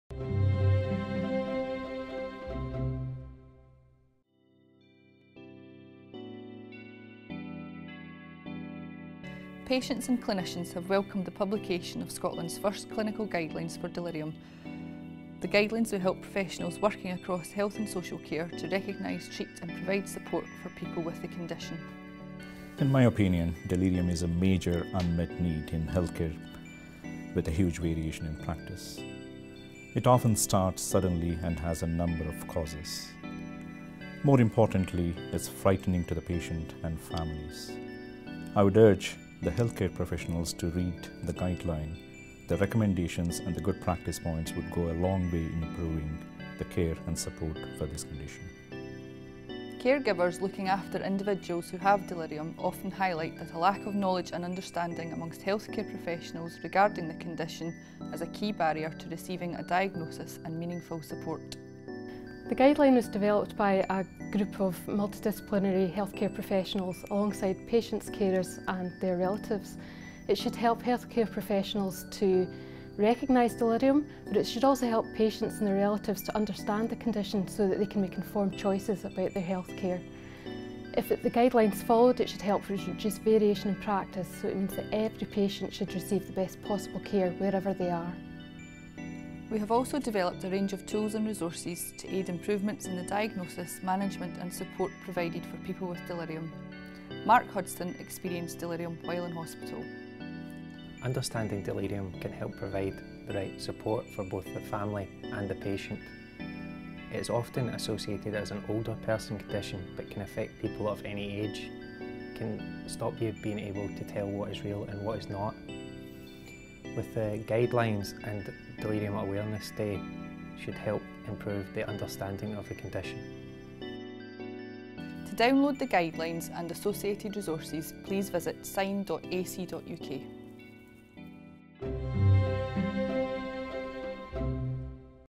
vnews: First Delirium Clinical Guidelines for Scotland Published on World Delirium Awareness Day
We’ve published Scotland’s first clinical guidelines for delirium to help health and social care professionals recognise, treat and provide support for people with delirium: